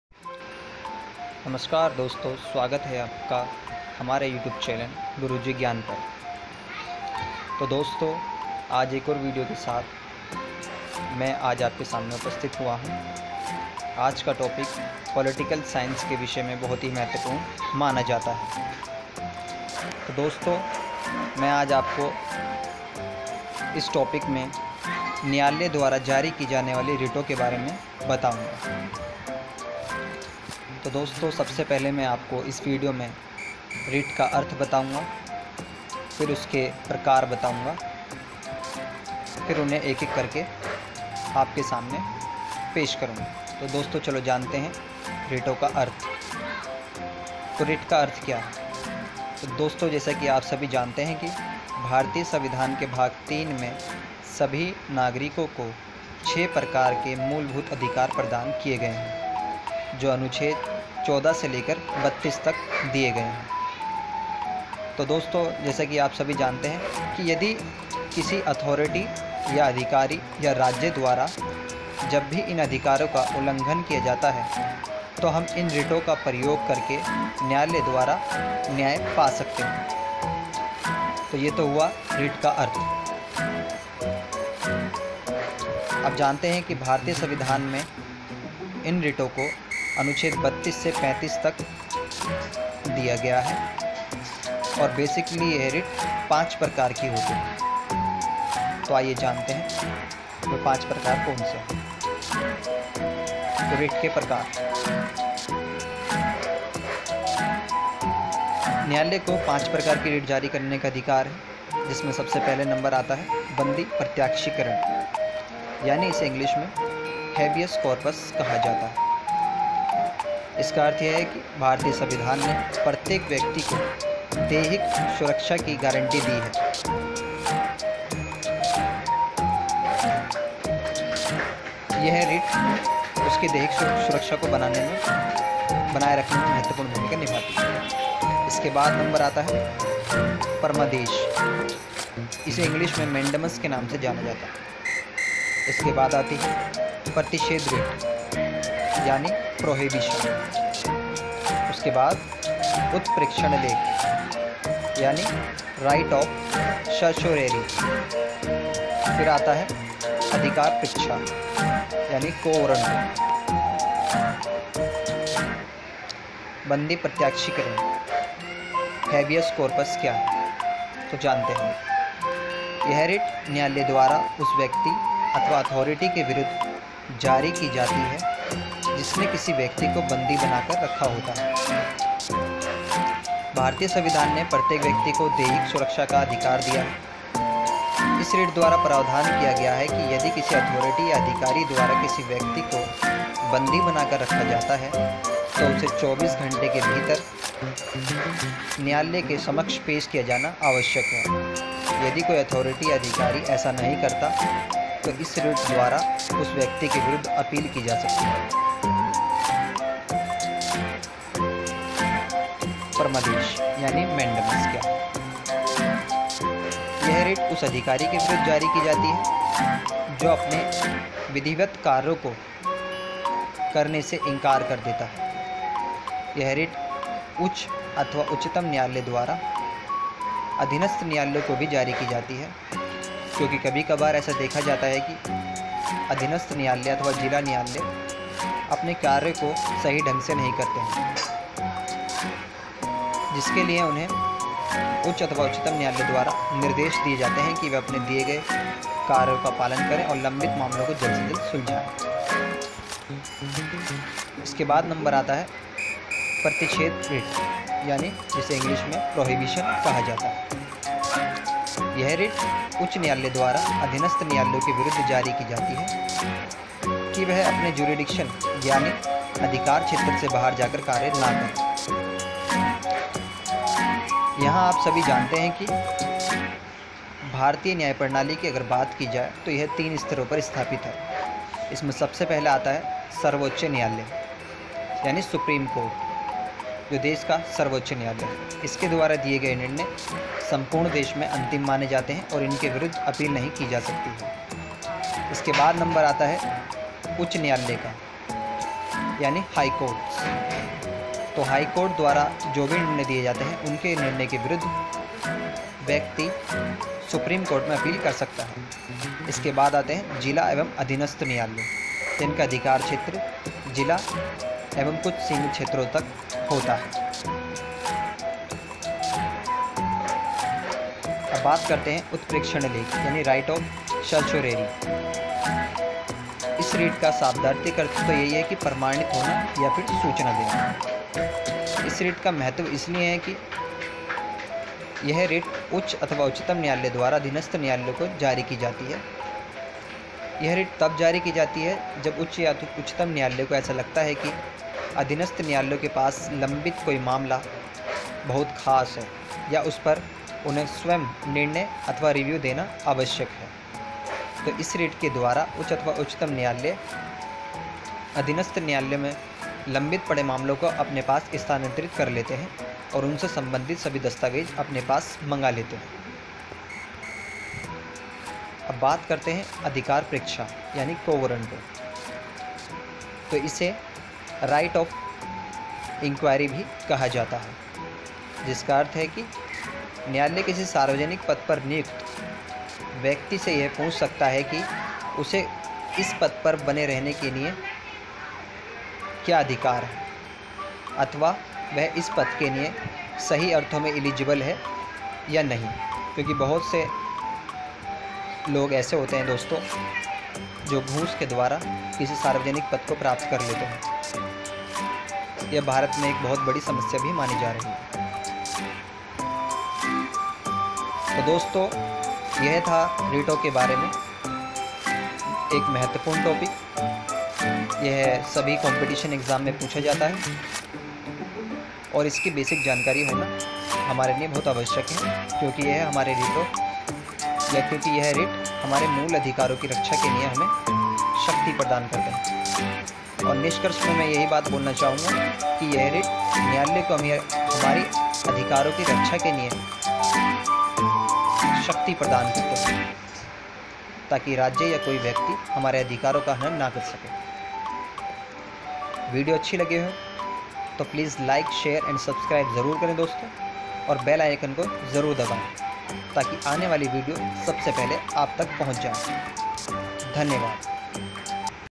Judicial writ
रिट के 5 प्रकार  TYPES OF WRIT FULL EXPLAINED IN HINDI 2021-SSC /RAILWAY/UPSC /STATE PCS IMPORTANT FOR ALL COMPETITION EXAM

FULL EXOLAINES
MEANING OF WRIT
5TYPE OF WRIT
HABIOUS CORPUS
MANDAMUS

WRIT OF CERTIORORI
WRIT OF PROHIBITION
QUO WARRANTO